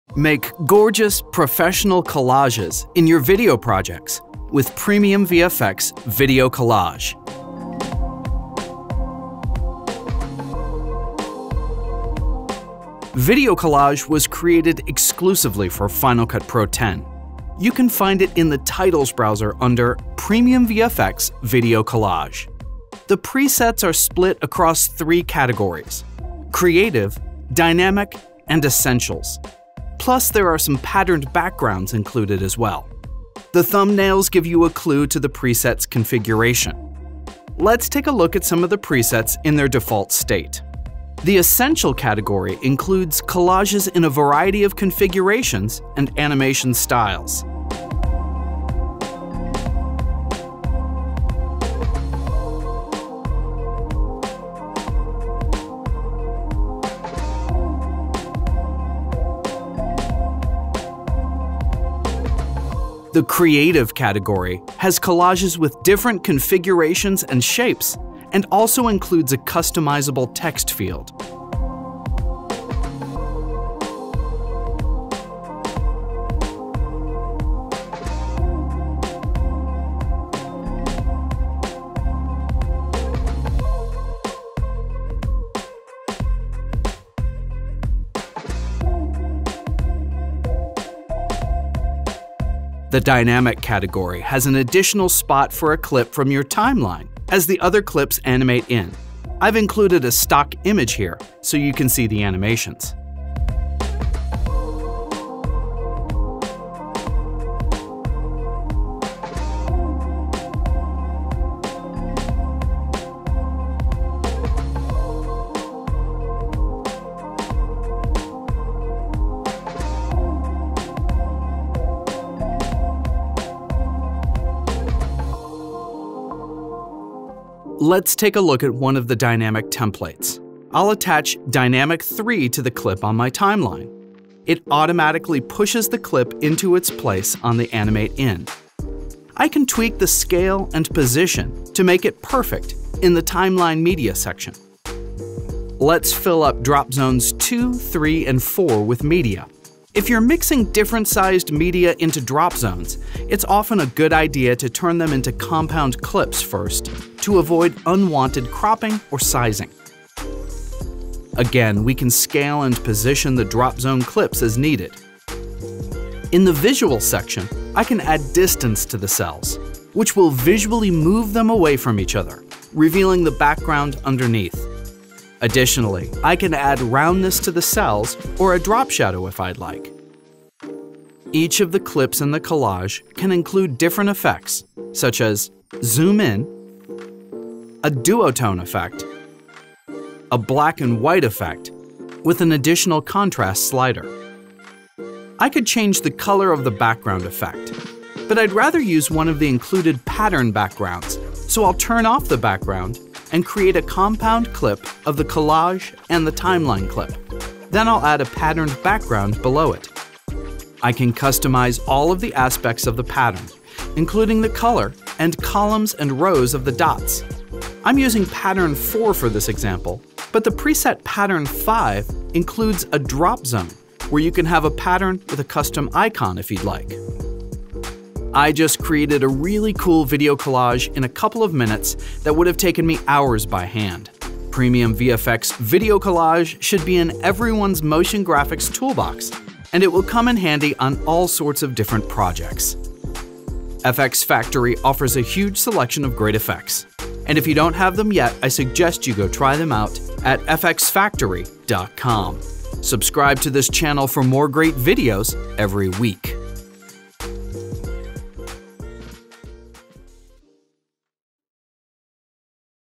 Video Collage Effect Tutorial for FCP
Make gorgeous, professional collages in your video projects with PremiumVFX Video Collage.